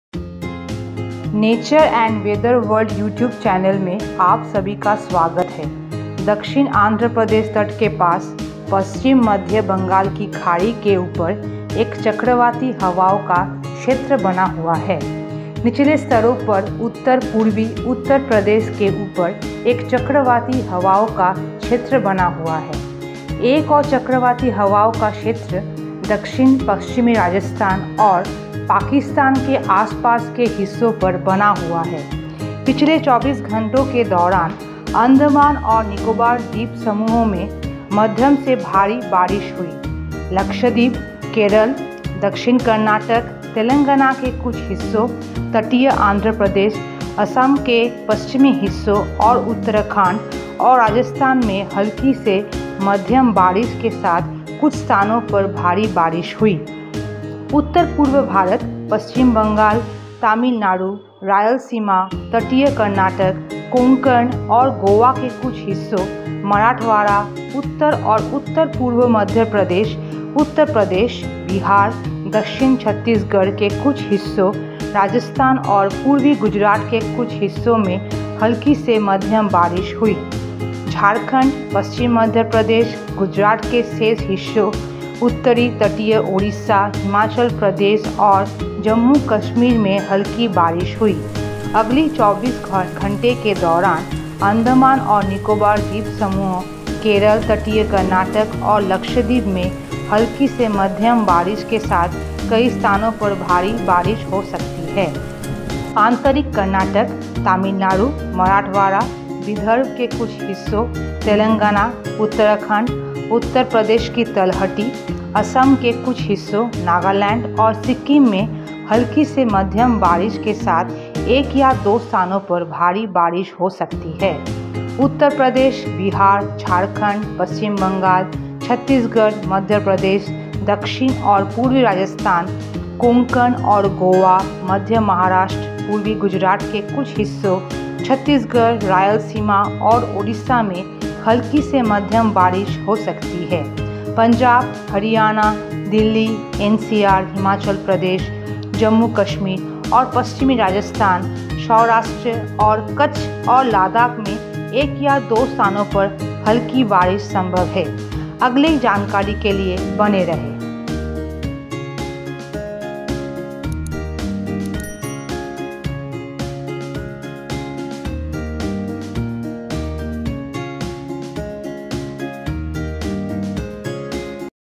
Weather Update : सम्पूर्ण भारत का मौसम पूर्वानुमान | Monsoon 2022
दक्षिण आंध्र प्रदेश तट के पास पश्चिम-मध्य बंगाल की खाड़ी के ऊपर एक चक्रवाती हवाओं का क्षेत्र बना हुआ है।

निचले स्तरों पर उत्तर पूर्वी उत्तर प्रदेश के ऊपर एक चक्रवाती हवाओं का क्षेत्र बना हुआ है।

एक और चक्रवाती हवाओं का क्षेत्र दक्षिण पश्चिमी राजस्थान और पाकिस्तान के आसपास के हिस्सों पर बना हुआ है।

पिछले 24 घंटों के दौरान, अंडमान और निकोबार द्वीप समूह में मध्यम से भारी बारिश हुई।

लक्षद्वीप, केरल, दक्षिण कर्नाटक, तेलंगाना के कुछ हिस्सों, तटीय आंध्र प्रदेश, असम के पश्चिमी हिस्सों और उत्तराखंड और राजस्थान में हल्की से मध्यम बारिश के साथ कुछ स्थानों पर भारी बारिश हुई।

उत्तर पूर्व भारत, पश्चिम बंगाल, तमिलनाडु, रायलसीमा, तटीय कर्नाटक, कोंकण और गोवा के कुछ हिस्सों, मराठवाड़ा, उत्तर और उत्तर पूर्व मध्य प्रदेश, उत्तर प्रदेश, बिहार, दक्षिण छत्तीसगढ़ के कुछ हिस्सों, राजस्थान और पूर्वी गुजरात के कुछ हिस्सों में हल्की से मध्यम बारिश हुई।

झारखंड, पश्चिम मध्य प्रदेश, गुजरात के शेष हिस्सों, उत्तरी तटीय ओडिशा, हिमाचल प्रदेश और जम्मू कश्मीर में हल्की बारिश हुई।

अगले 24 घंटों के दौरान, अंडमान और निकोबार द्वीप समूह, केरल, तटीय कर्नाटक और लक्षद्वीप में हल्की से मध्यम बारिश के साथ कई स्थानों पर भारी बारिश हो सकती है।

आंतरिक कर्नाटक, तमिलनाडु, मराठवाड़ा, विदर्भ के कुछ हिस्सों, तेलंगाना, उत्तराखंड, उत्तर प्रदेश की तलहटी, असम के कुछ हिस्सों, नागालैंड और सिक्किम में हल्की से मध्यम बारिश के साथ एक या दो स्थानों पर भारी बारिश हो सकती है।

उत्तर प्रदेश, बिहार, झारखंड, पश्चिम बंगाल, छत्तीसगढ़, मध्य प्रदेश, दक्षिण और पूर्वी राजस्थान, कोंकण और गोवा, मध्य महाराष्ट्र, पूर्वी गुजरात के कुछ हिस्सों, छत्तीसगढ़, रायलसीमा और ओडिशा में हल्की से मध्यम बारिश हो सकती है।

पंजाब, हरियाणा, दिल्ली एनसीआर, हिमाचल प्रदेश, जम्मू कश्मीर और पश्चिमी राजस्थान, सौराष्ट्र और कच्छ और लद्दाख में एक या दो स्थानों पर हल्की बारिश संभव है।

আলিপুর আবহাওয়ার খবর

সকলকে ধন্যবাদ জানাই সাথে থাকার জন্য । সব আপডেট নিয়মিত পেতে চ্যানেলটি নজরে রাখুন 🙏
Analysis and Forecasting of Weather